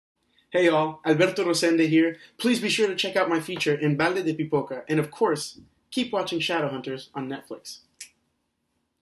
Entrevista com Alberto Rosende - Balde de Pipoca
Conversamos com Alberto sobre a evolução e transformação de Simon na série, o carinhos dos fãs brasileiros, sobre o elenco da série muito mais! Confira